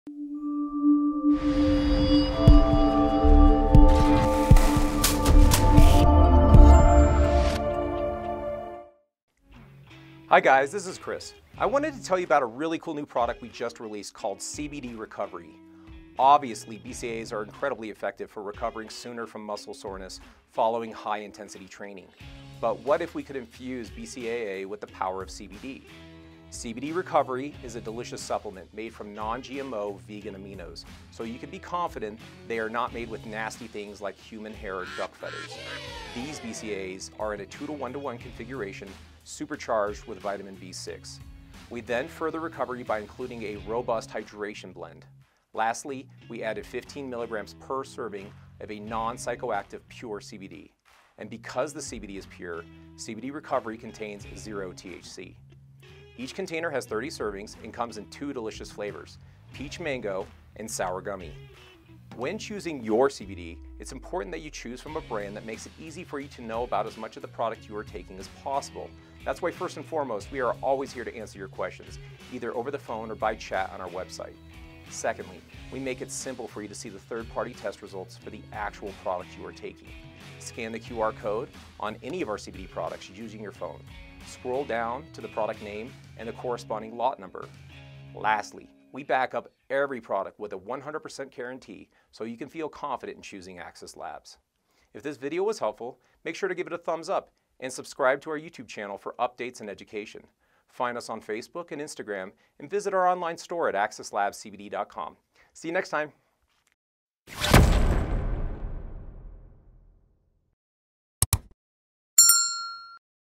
Elevate Your Workout Recovery With CBD+BCAA | Axis Labs
Take active recovery to new levels with our excellent CBD+BCAA supplements. Made with plant-based aminos and our cold-extract CBD, anyone can enjoy this recovery product, even those on vegan diets!

Did your last workout leave you feeling fatigued? Crushed? Just worn out? Stay alert, hydrated, and ready to hit the gym with our CBD+BCAA recovery supplements!

With a 2:1:1 BCAA ratio, and using our pure CBD extracts, our premium recovery powder helps you recover from exercise more quickly, and ensures that your gains are here to last. We’re excited to offer this product in two great tasting flavors — Peach Mango and Sour Gummy.